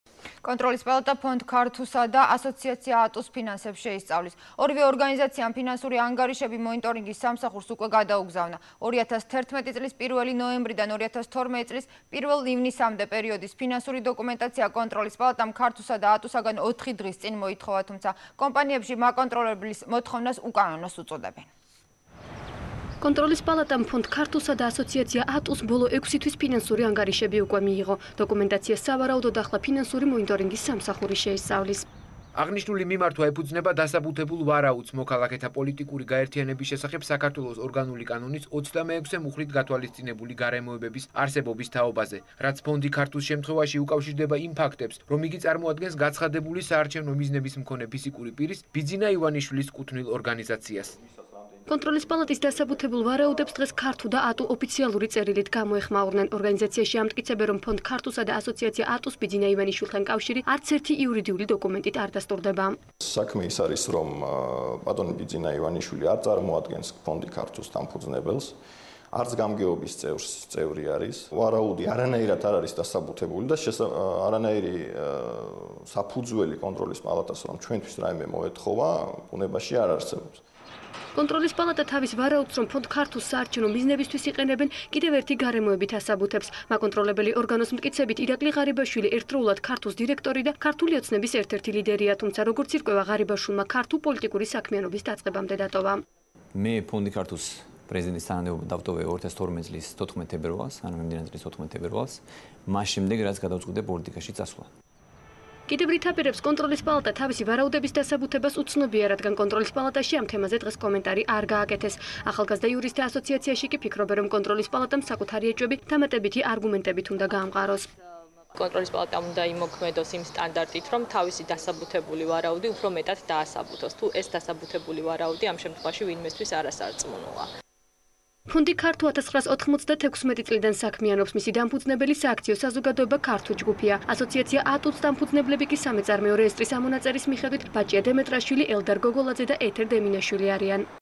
ახალი 9 | ქართუ და ატუ | 12.06.12
კონტროლის პალატა ფონდ "ქართუსა" და ასოციაცია "ატუს" ფინანსებს შეისწავლის. ორივე ორგანიზაციამ ფინანსური ანგარიშები მონიტორინგის სამსახურს უკვე გადაუგზავნა. 2011 წლის 1 ნოემბრიდან  2012 წლის 1 ივნისამდე პერიოდის ფინანსური დოკუმენტაცია კონტროლის პალატამ "ქართუსა" და "ატუსგან" 4 დღის წინ მოითხოვა, თუმცა კომპანიებში მაკონტროლებლის მოთხოვნას უკანონოს უწოდებენ.